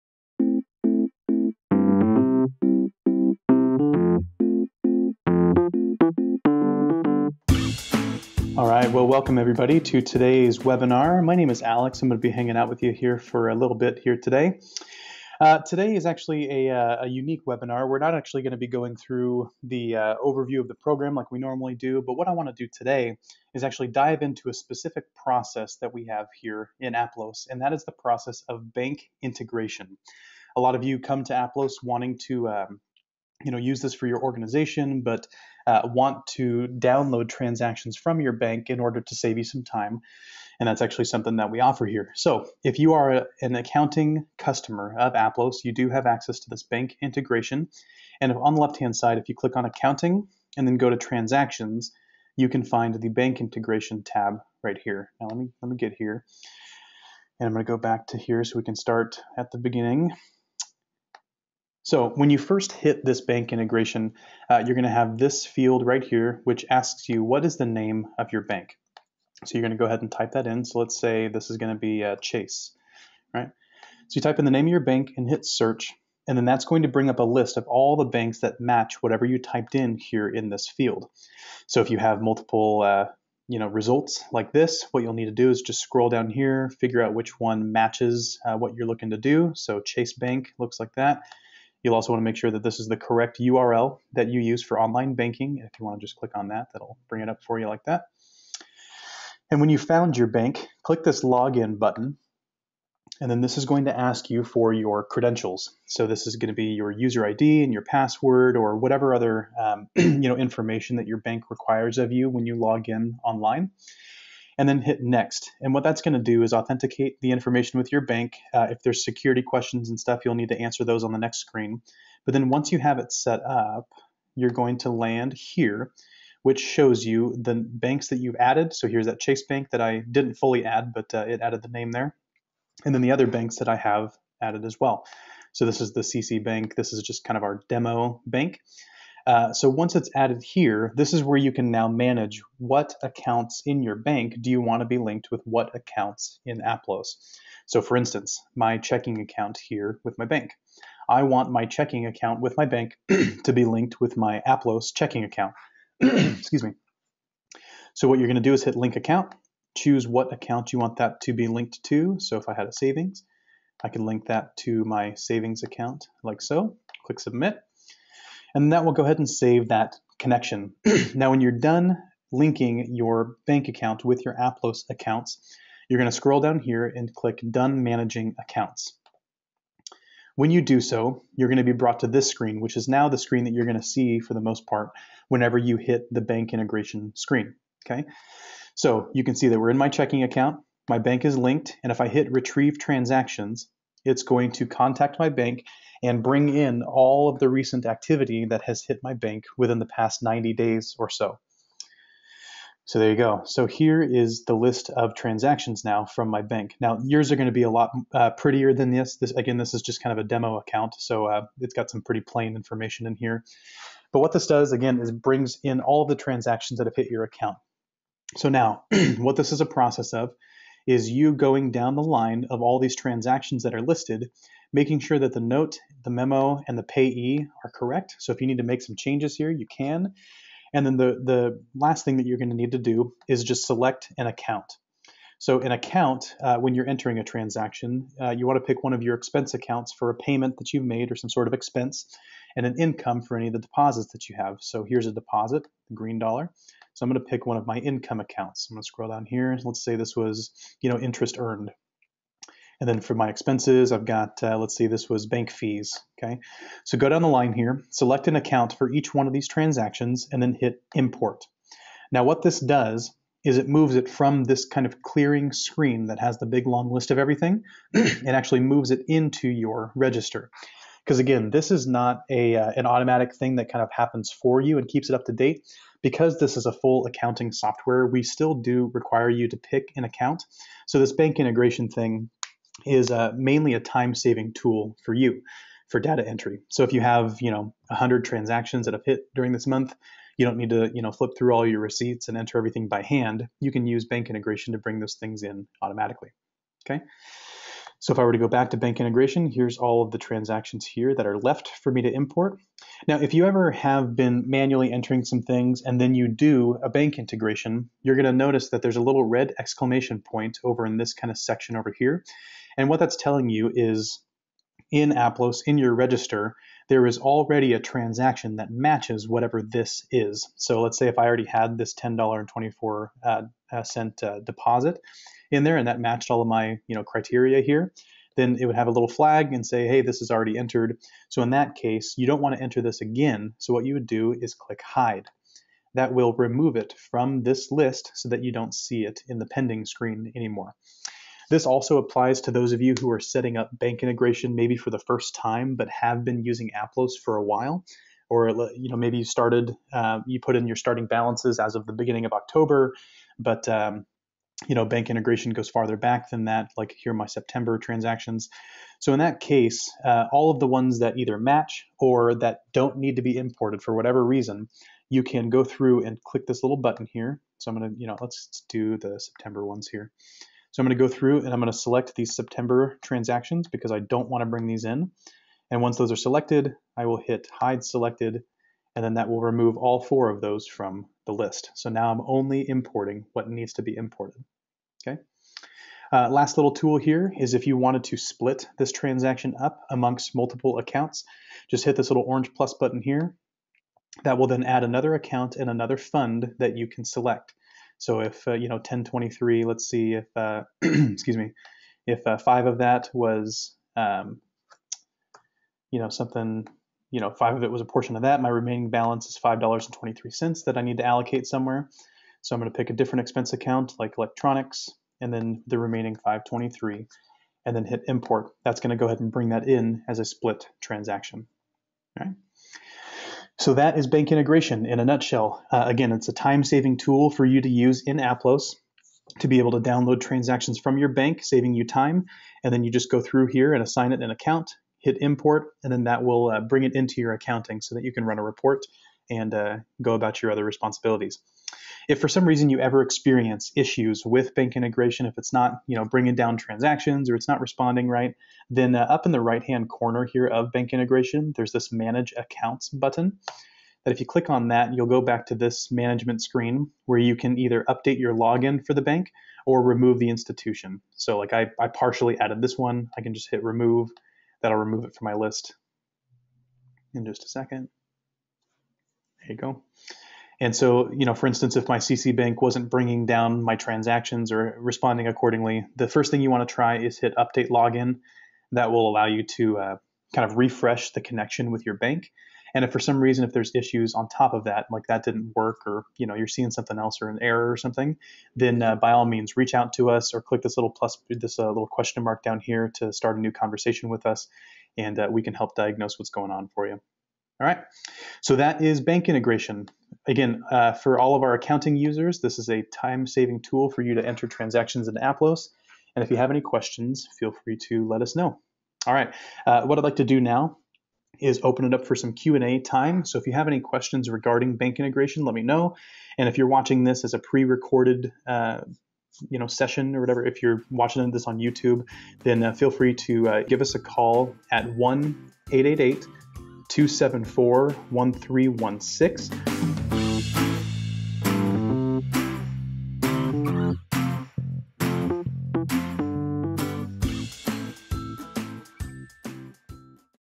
How To Use Bank Integration | Aplos Webinar
Are you interested in learning how to use bank integration in Aplos?  Check out this recorded webinar to see how to manage your accounting faster by importing your transactions directly from your bank into Aplos Accounting.